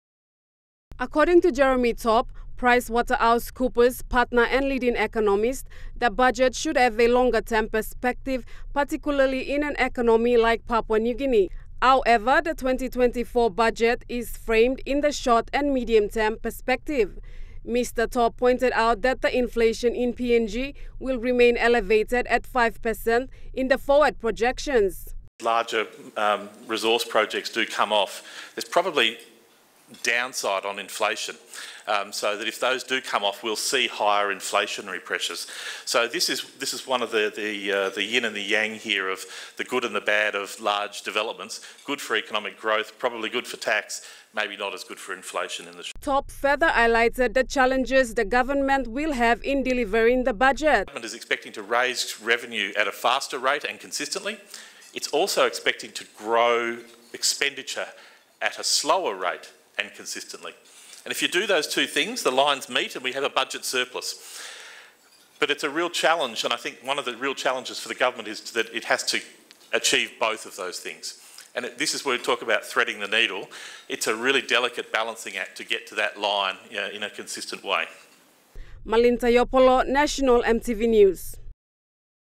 BUDGET BREAKFAST
The annual Budget Breakfast was held at the Hilton Hotel for the presentation of the 2024 budget by the Treasurer, Ian Ling-Stuckey followed by an Economic analysis of the budget.
Treasurer Ling-Stucky however did not turn up, which saw the event proceed straight into the economic analysis.